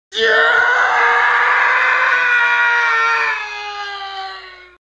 This Voki but the audio is replaced
scsrciciripitpt:

this Voki's audio was replaced with something else! hooray!

English, Adult Voices, Tessa (South African).

Background, Outdoors, Colorful Pond.
Character, Animals, Capuchin Monkey.